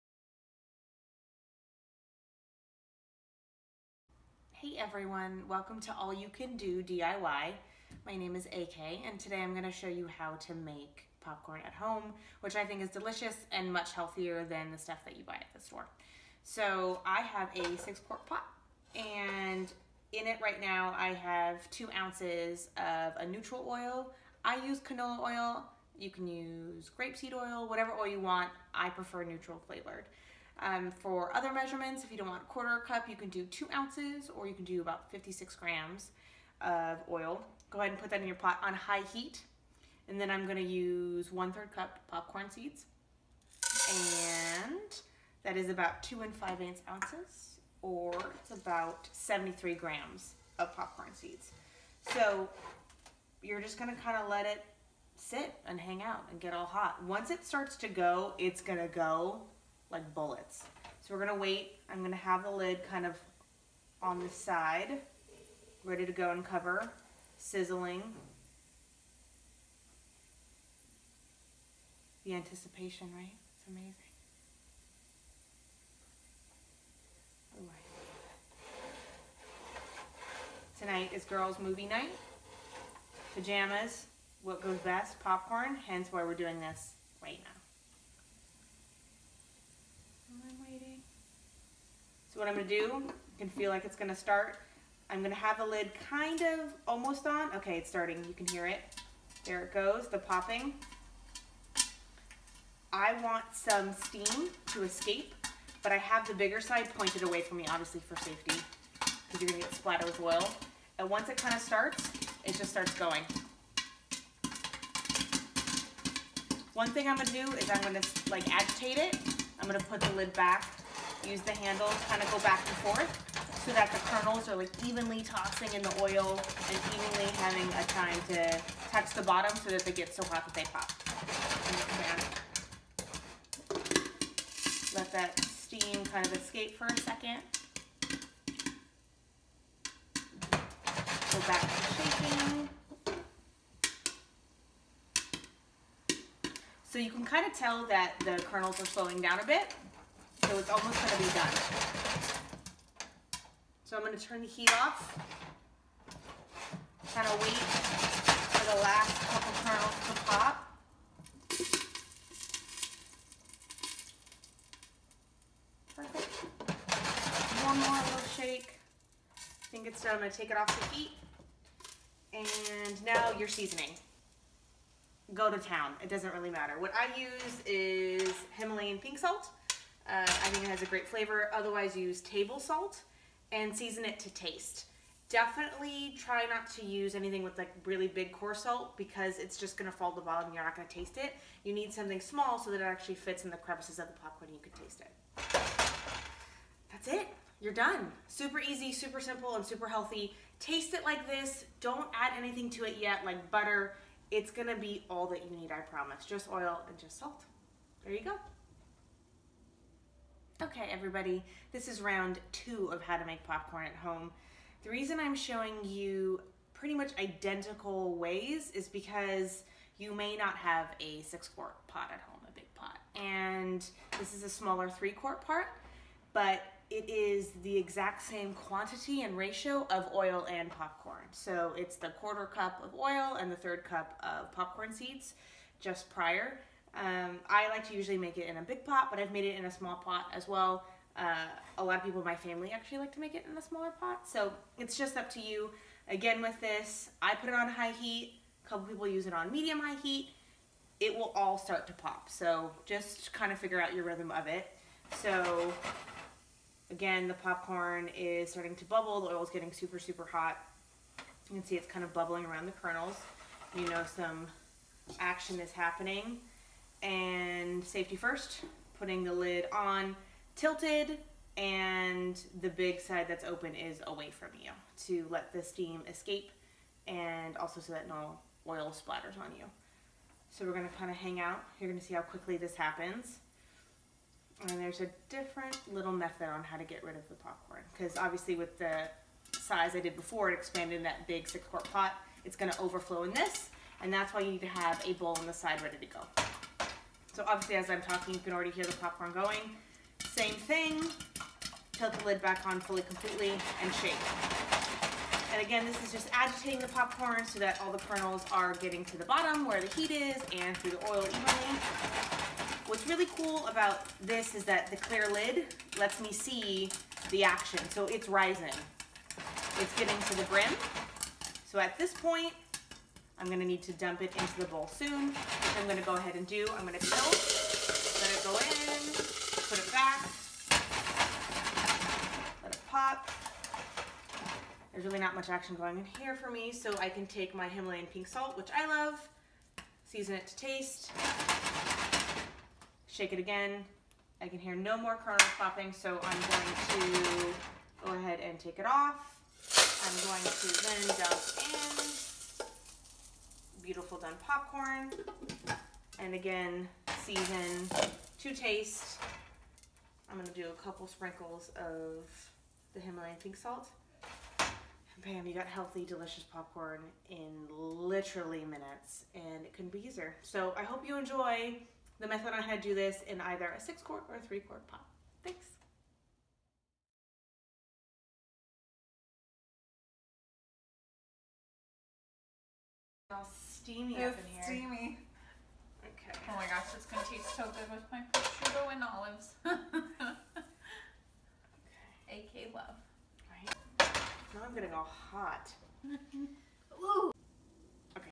How to video on making healthy Popcorn at home on the stovetop.
How to video on making healthy Popcorn at home on the stovetop with 3 simple ingredients!